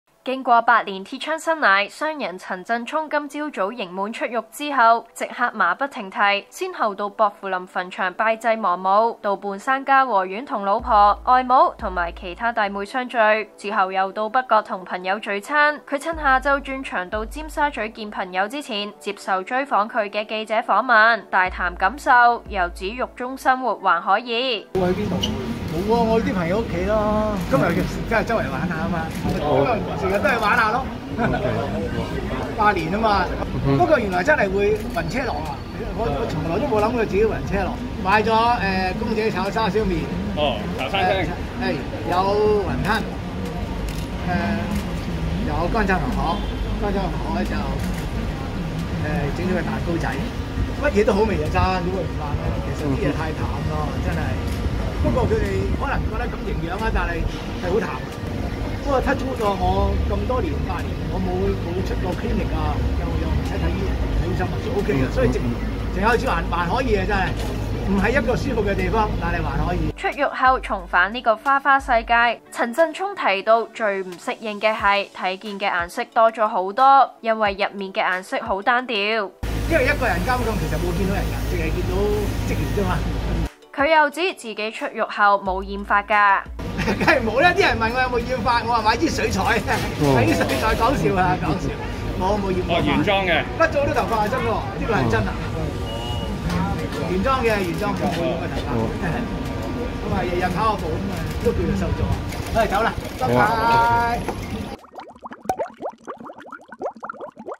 指獄中生活還可以 陳振聰：入面唔係一個舒服地方
出獄後重回花花世界，陳振聰提到最感不適應的，是看見的顏色多了很多。